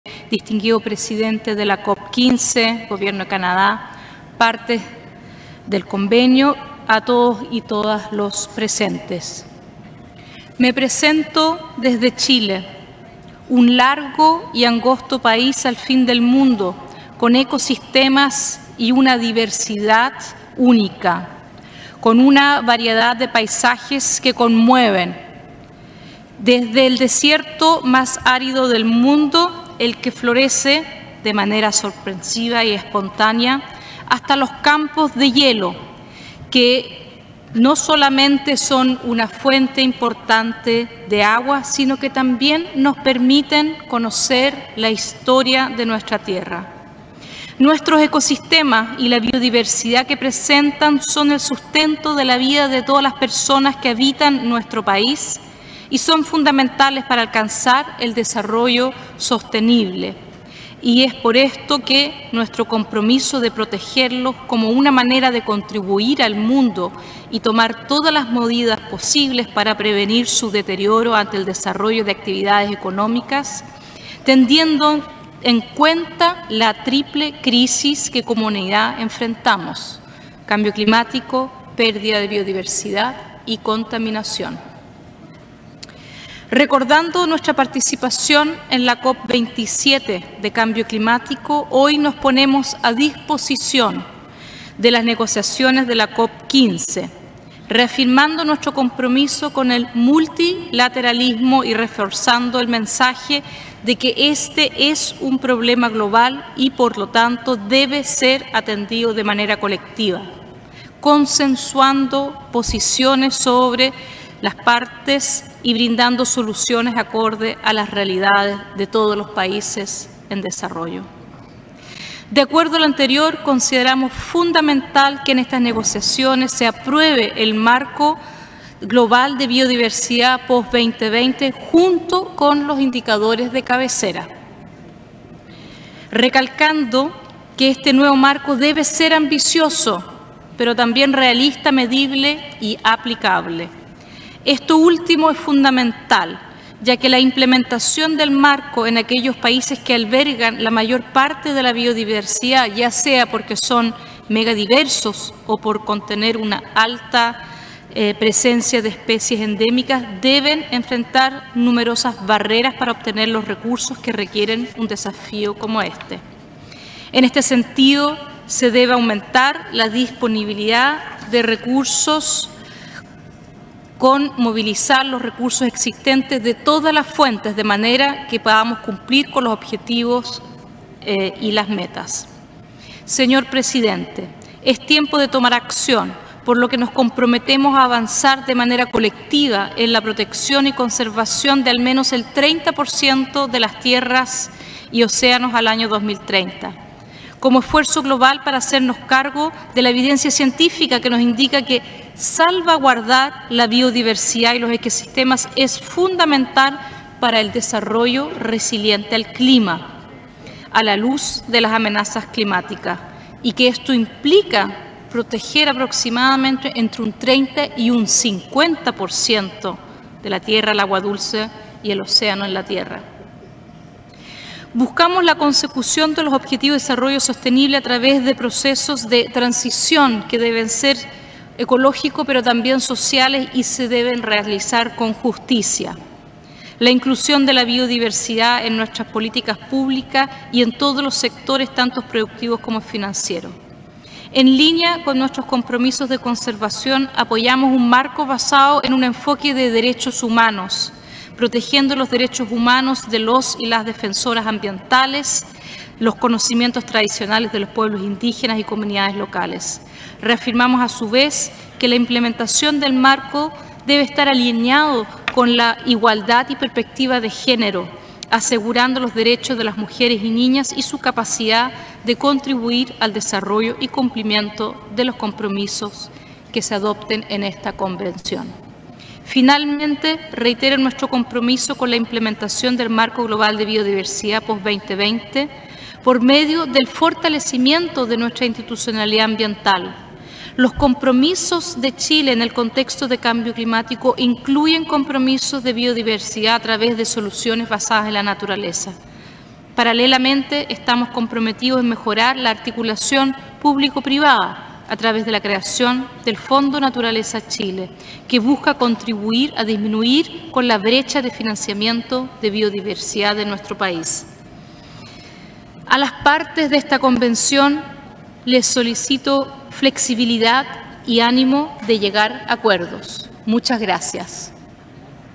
Intervención ministra Maisa Rojas COP15
Intervención ministra Maisa Rojas en Segmento de Alto Nivel de la COP15 de Biodiversidad, en Canadá. - Ministerio del Medio Ambiente